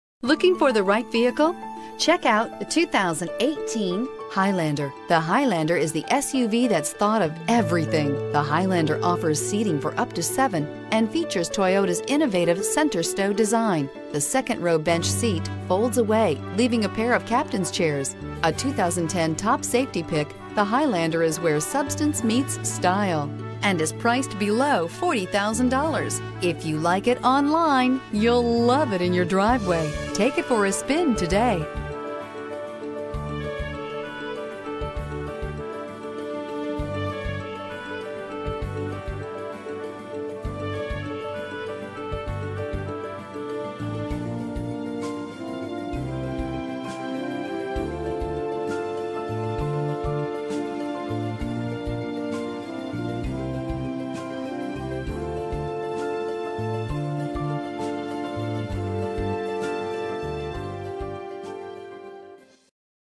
2018 Toyota Highlander Elk Grove Toyota 117488
2018 Toyota Highlander  - Stock#: 117488 - VIN#: 5TDBZRFH6JS805371

Check out this 2018 Toyota Highlander.   Featuring 8-speed Automatic transmission, it has 0 miles.  The Special Internet Price is $35284. Contact the dealership today.